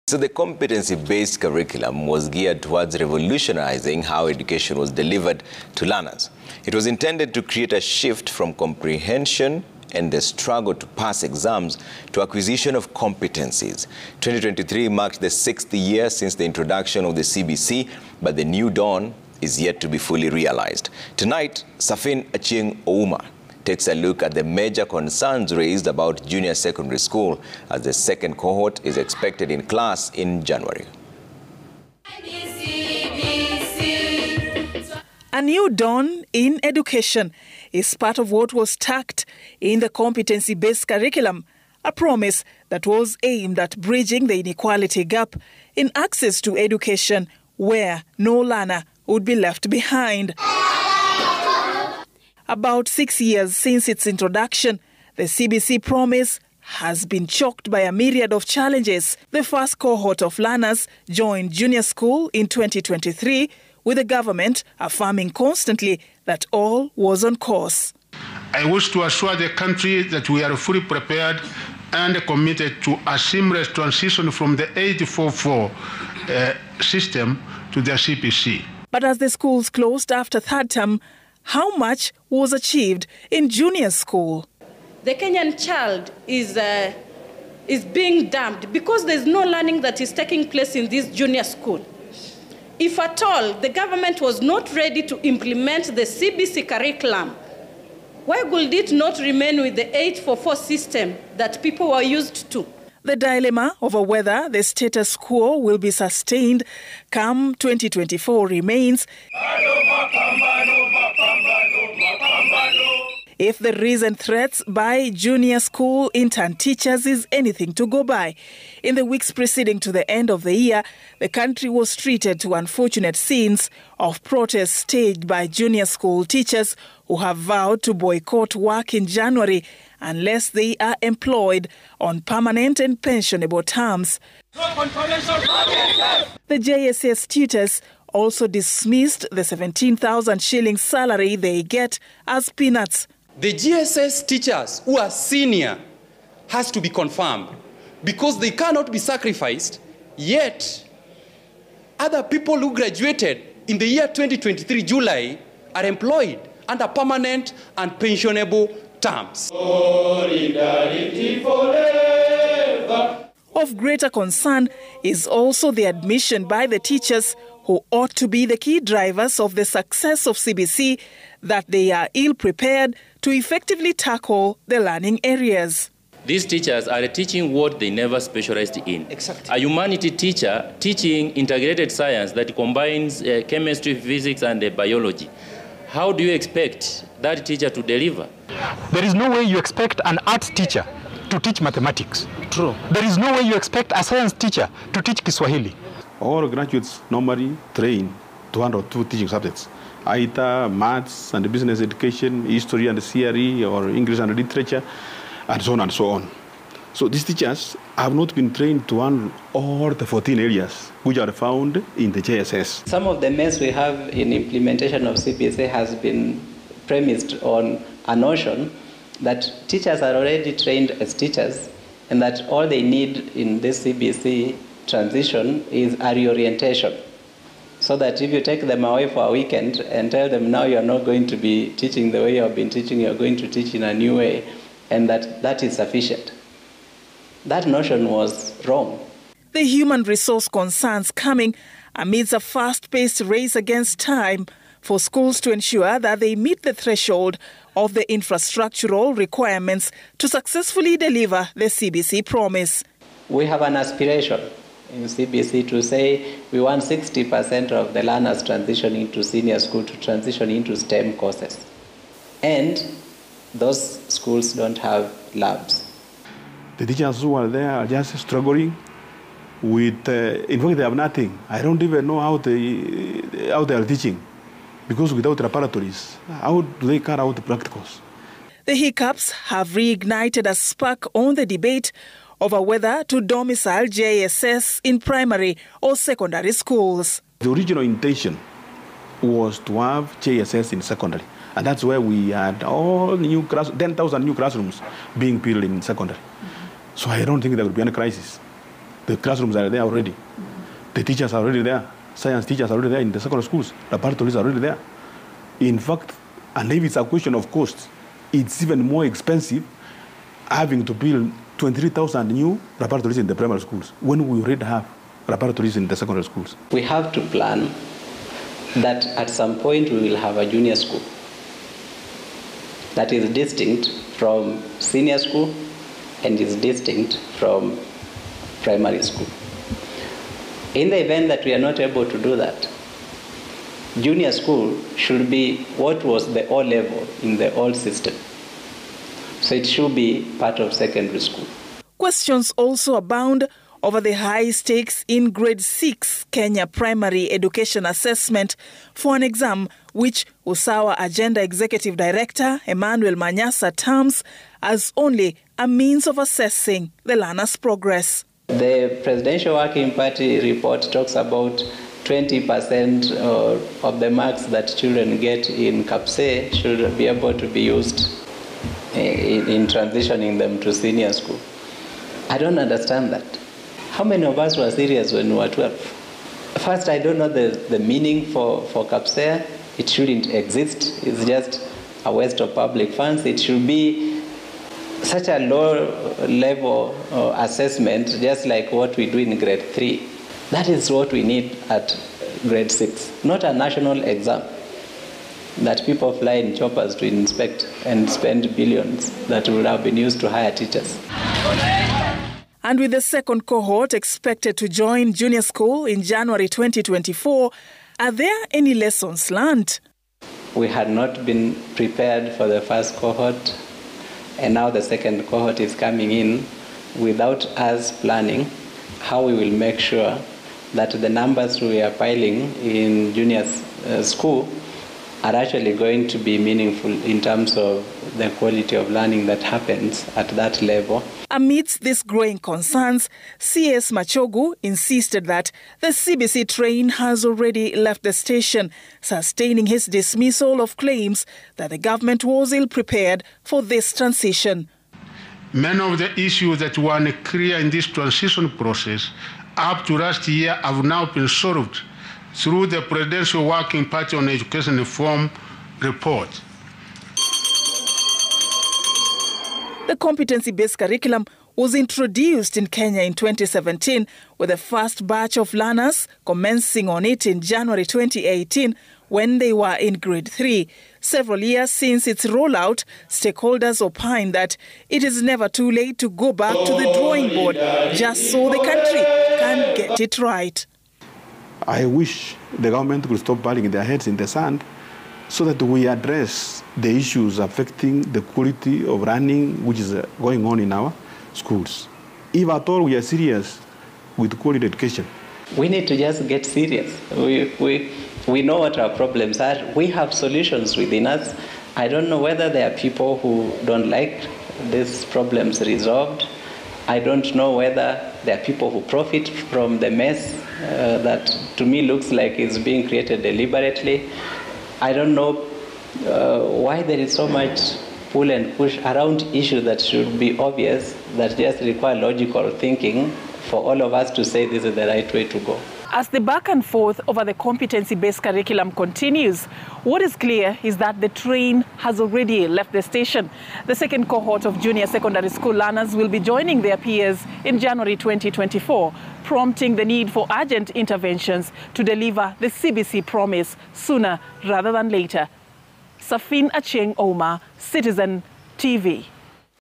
THE CBC CHALLENGES: Many public schools face shortage of trained JSS teachers
The Competency Based Curriculum was geared towards revolutionising how education was delivered to learners. It was intended to create a shift from comprehension and the struggle to pass exams to acquisition of competencies. 2023 marked the 6th year since the introduction of CBC but the new dawn is yet to be fully realised. Tonight Serfine Achieng’ Ouma takes a look at the major concerns raised about junior secondary school as the second cohort is expected in class in January.